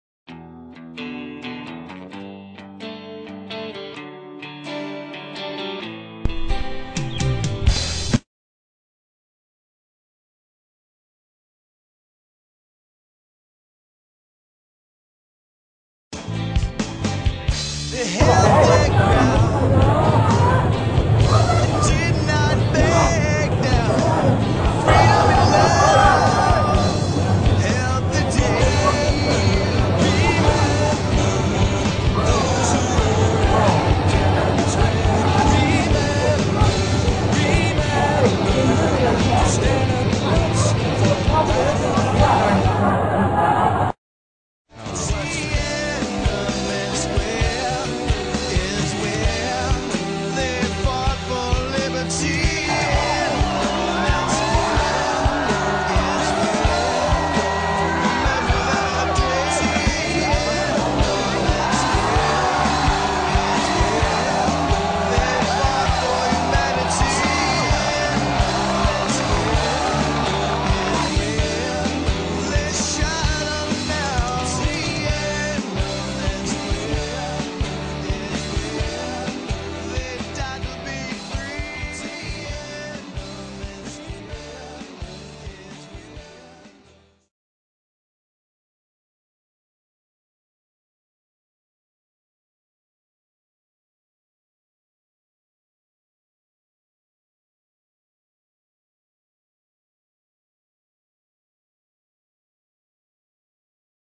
Died for Freedom:
about the Tiananmen Square protests in the 1980's.